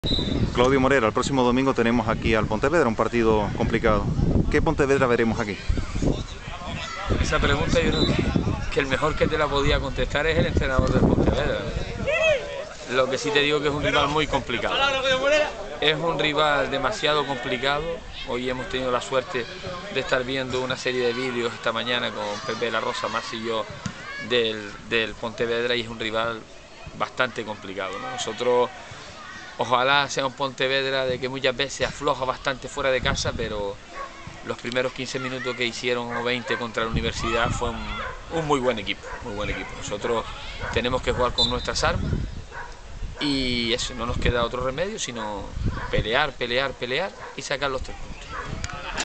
Declaraciones de Claudio Morera sobre el Pontevedra
Morera comenta el próximo encuentro del Villa